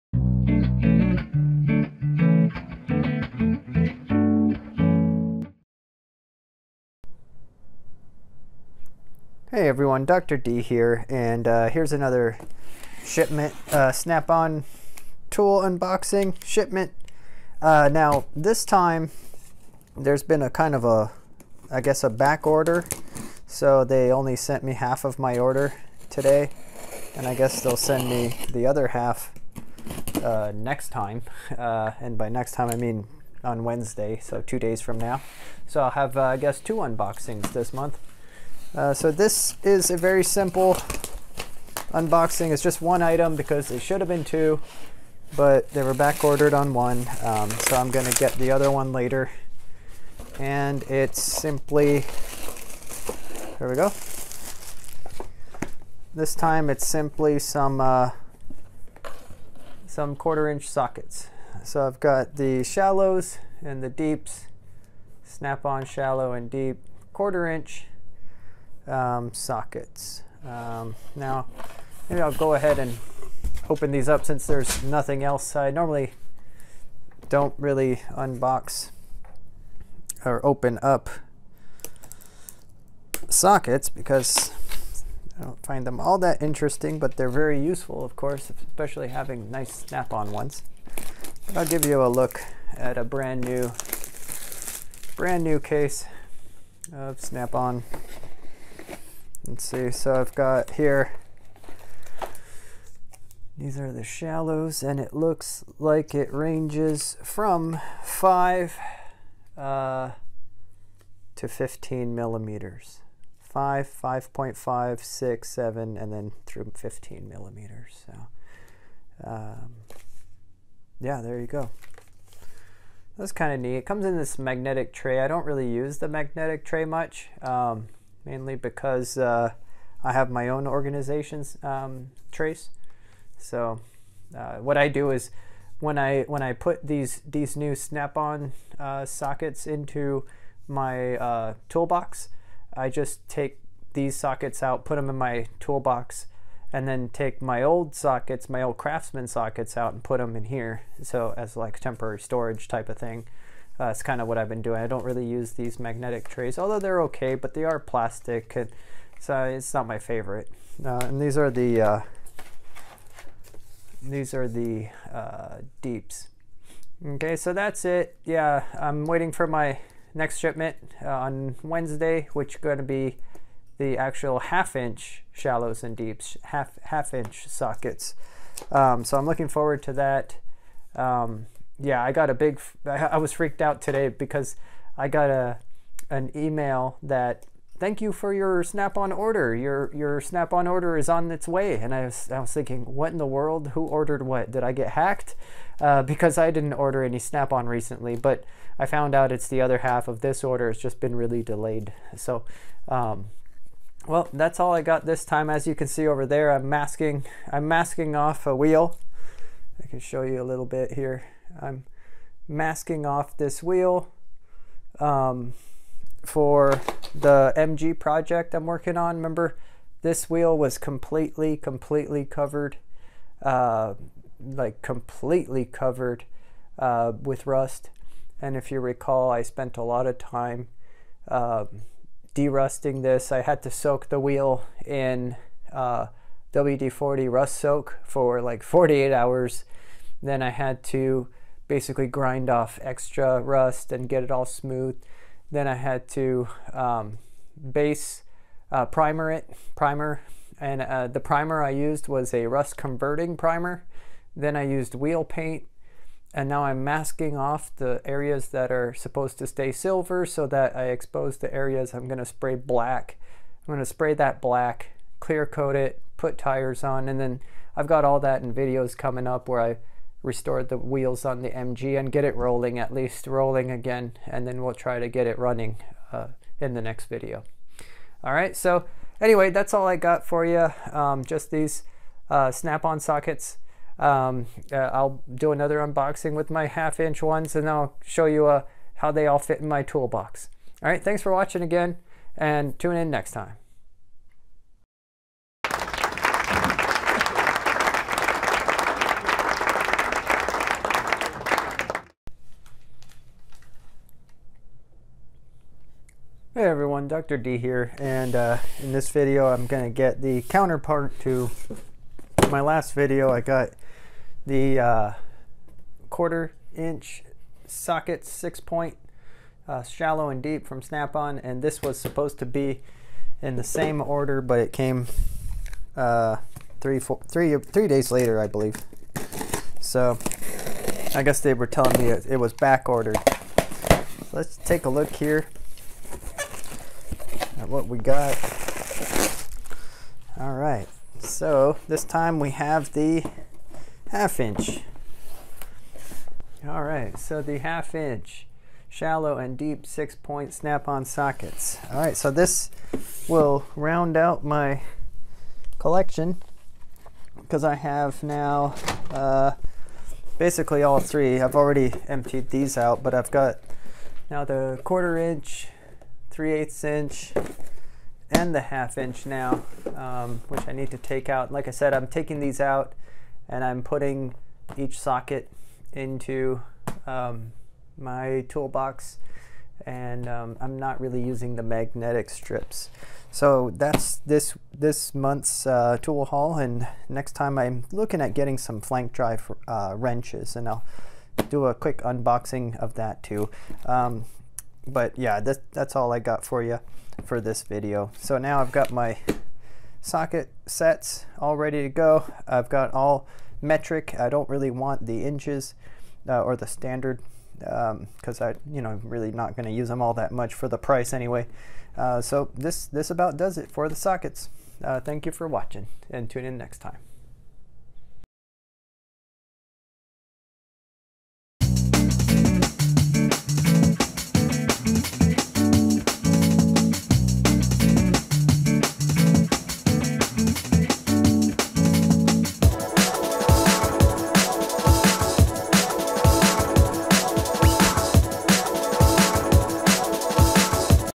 Snap-on tool haul for Feb. 2019 - Sockets!
Here I unbox my 1/4 inch and 1/2 inch metric shallow and deep six point sockets from Snap-on.